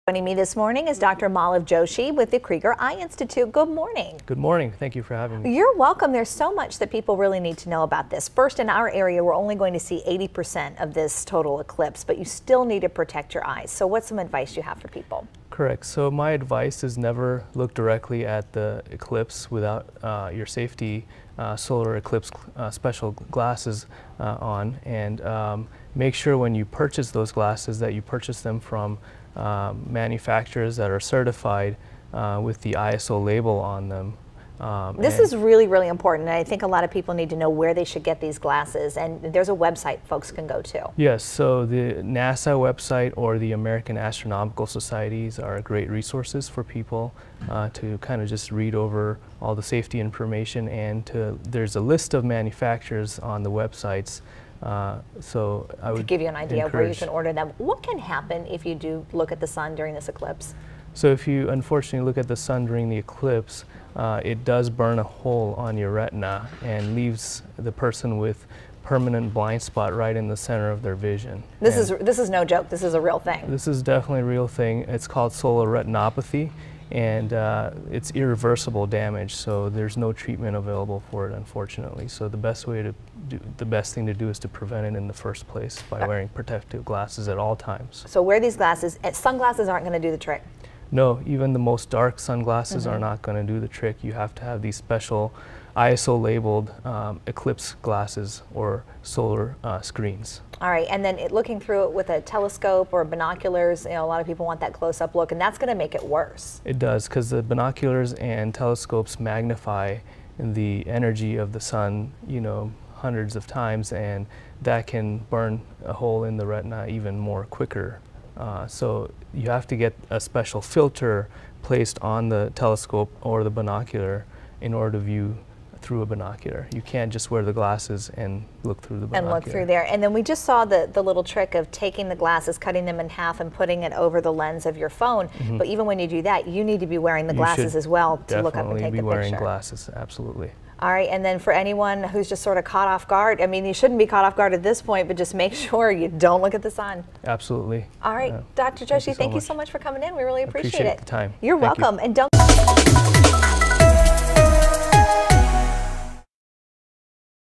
Video: Doctor explains how viewing eclipse can cause permanent eye damage
Dr. Malav Joshi with the Kriger Eye Institute explains how viewing eclipse can cause permanent eye damage